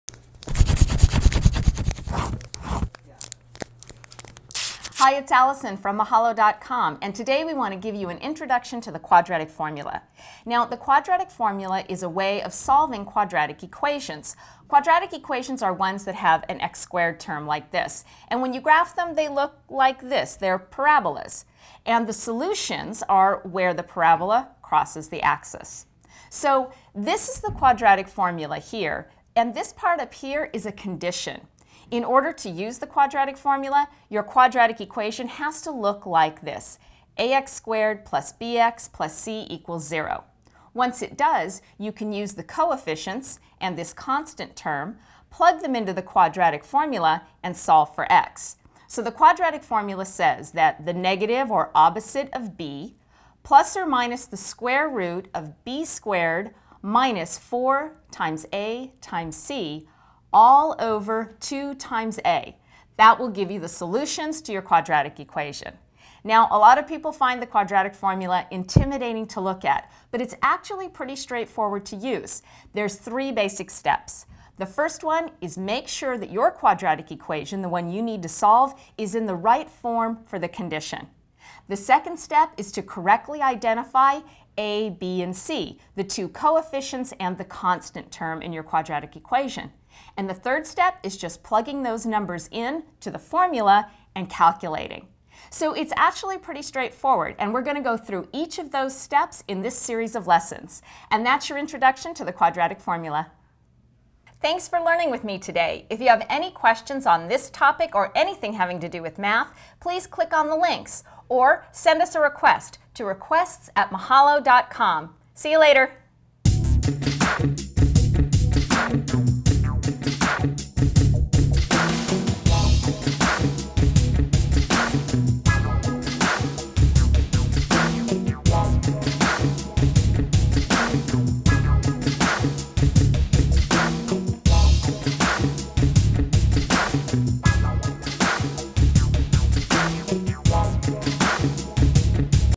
The Quadratic Formula - An Introduction
Learn about the quadratic formula with Allison Moffett, host of the Mahalo Math Channel. Ms. Moffett uses her years of experience in the mathematics field to introduce the formula in an understandable manner. She explains what the formula is used for and what the terms related to it mean. For more math tutorials, visit the Mahalo.com YouTube channel.

The quadratic formula is a way of solving
quadratic
equations. Quadratic equations are those which contain an x-squared term.  The quadratic equation, when placed on a graph, is a parabola and the solution to the quadratic formula defines where the parabola
crosses the axis.
In order to use the quadratic formula, the condition of
having the quadratic equation in the correct form must first be satisfied.
 Once the equation is in the correct format,

The Correct Quadratic Equation Form

The structure of the quadratic equation is critical when using the quadratic formula. In order to find the correct information to put in the quadratic formula, the quadratic equation must be in the correct format. The equation must be structured as a squared term plus an x term plus a constant. This structure must be equal to zero. The following is an example of a quadratic equation:

x^2 + x + 2 = 0

Aligning the terms correctly in preparation of using

In the quadratic equation ax^2 +bx + c = 0, the "a" and "b" are called
coefficients.  The "c" in the equation is a constant.  When using the quadratic formula to solve the equation, the coefficients and constants are used. It is important to determine the correct coefficients and constant in order to find a correct solution to the equation.
 Whether a coefficient or constant is positive or negative is determined by the sign of the portion of the equation it falls in. If the squared term is negative (-ax^2), the coefficient "a" is a negative number. Similarly, if the x term and the constant are subtracted (-bx-c), then coefficient "b" and constant "c" are negative.

When a quadratic equation contains a squared term or an x term with no coefficient shown, the coefficient is either -1 or +1, depending on the sign of the term.

The quadratic formula format is
x = -b±√-(b^2-4ac)
                                                             2a

To use the formula, the coefficients and constant are plugged in and calculations are made to solve for X.
 The quadratic formula is
actually two formulas in one.  Since the
sign preceding the square root is a "±" symbol, the formula must be calculated twice, once using